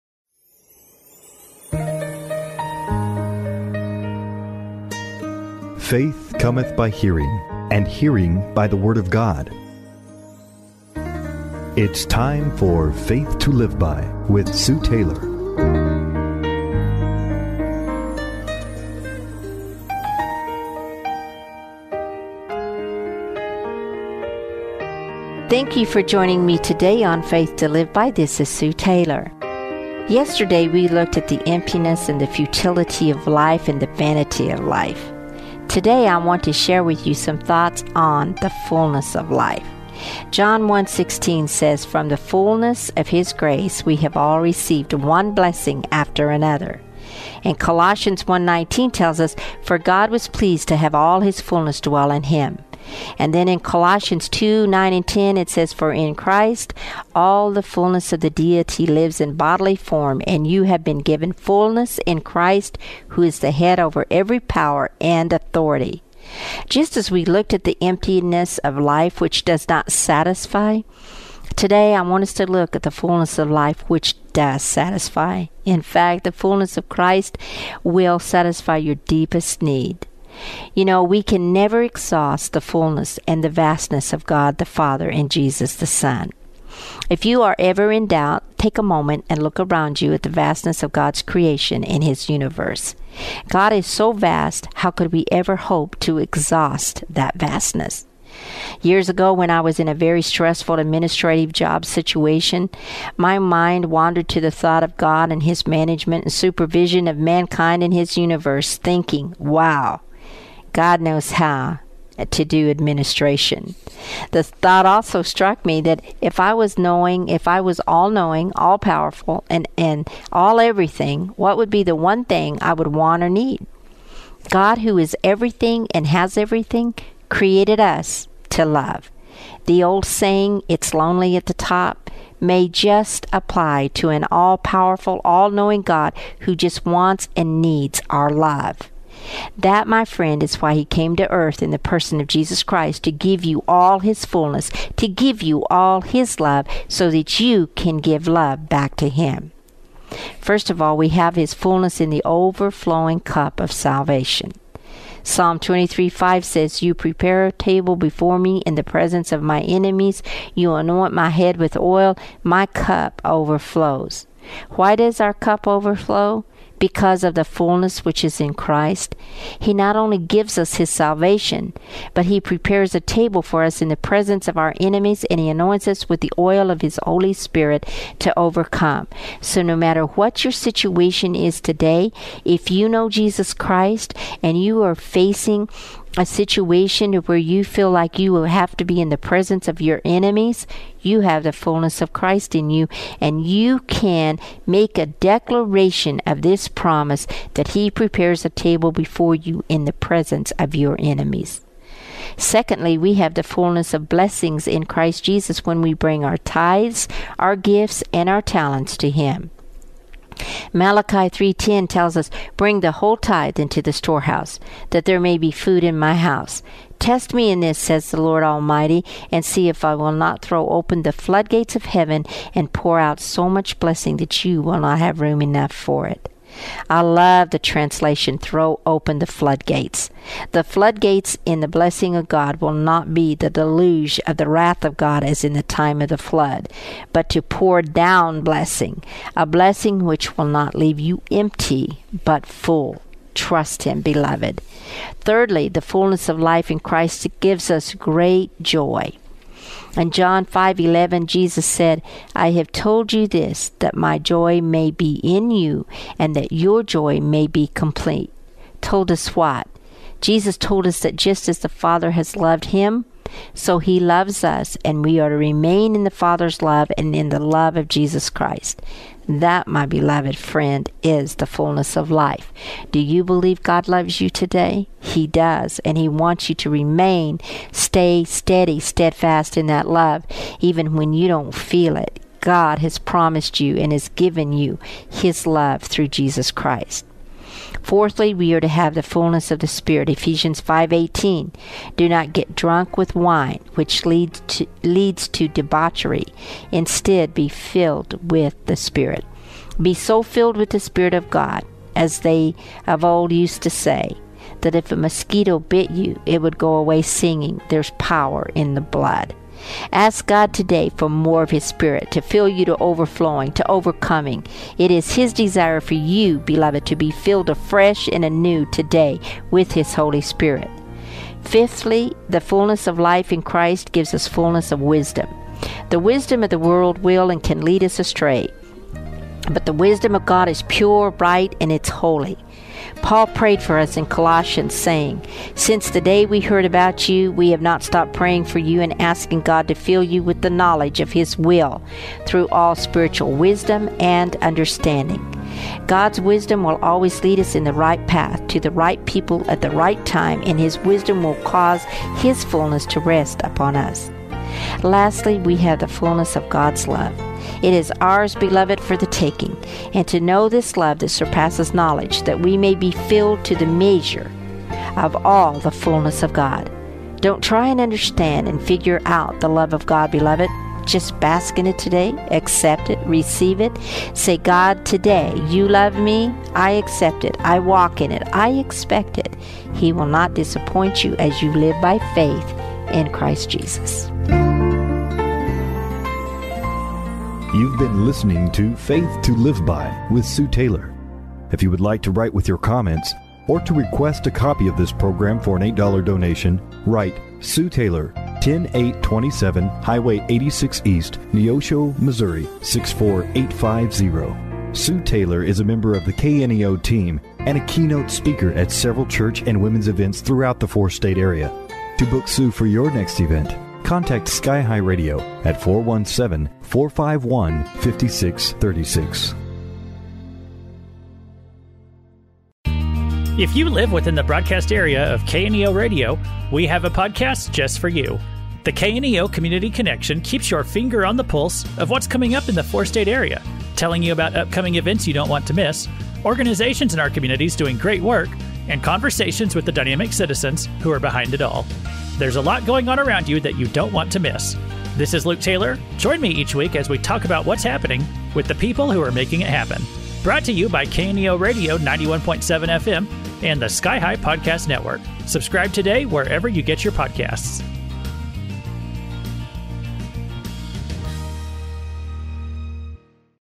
Fullness of Life (Part 2)
Today, Sue Taylor speaks on the fullness of life. Your Faith to Live By devotional thought for July 26, 2023.